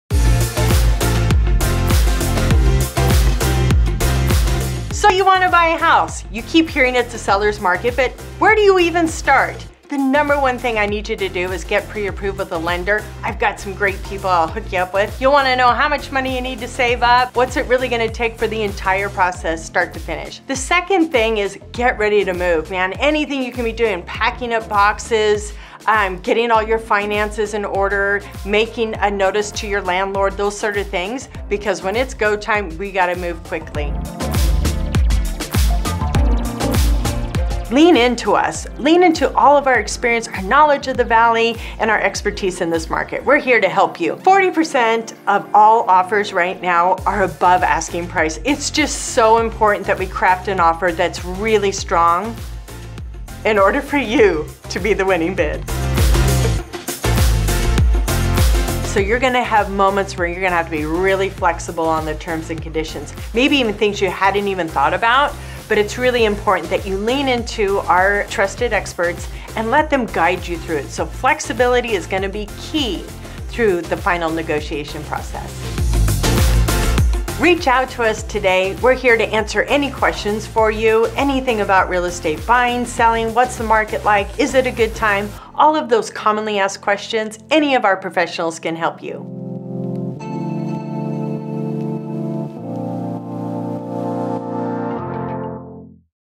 5 Tips for Making Your Best Offer
Making an offer on a home can be intimidating, but don't worry, we're here for you! Here are 5 great tips to remember when it's time to make an offer.